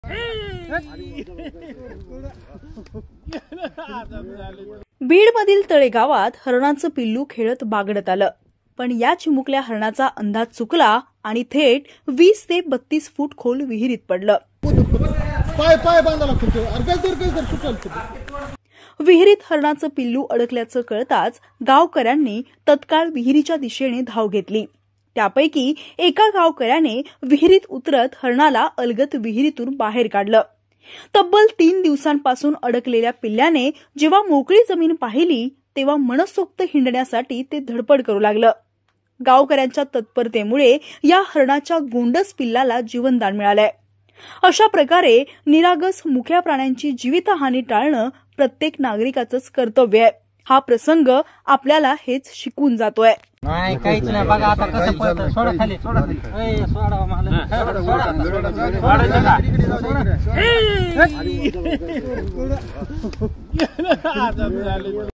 Beed Baby Deer Rescue | जीवाची बाजी लावत वाचवला हरणाच्या पिल्लाचा जीव | Maharashtra Times
बीडमधील तळेगावात हरणांचं पिल्लू खेळत बागडत आलं. पण या चिमुकल्या हरणाचा अंदाज चुकला आणि तो थेट 20 ते 32 फूट खोल विहिरीत पडला. विहरीत हरणाचं पिल्लू अडकल्याचं कळताच गावकऱ्यांनी तत्काळ विहरीच्या दिशेने धाव घेतली. त्यापैकी एका गावकऱ्याने विहरीत उतरत हरणाला अलगद  विहीरीबाहेर काढलं. तब्बल तीन दिवसांपासून अडकलेल्या पिल्ल्लाने जेव्हा मोकळी जमीन पाहीली तेव्हा मनसोक्त हींडण्यासाठी धडपड करु लागला.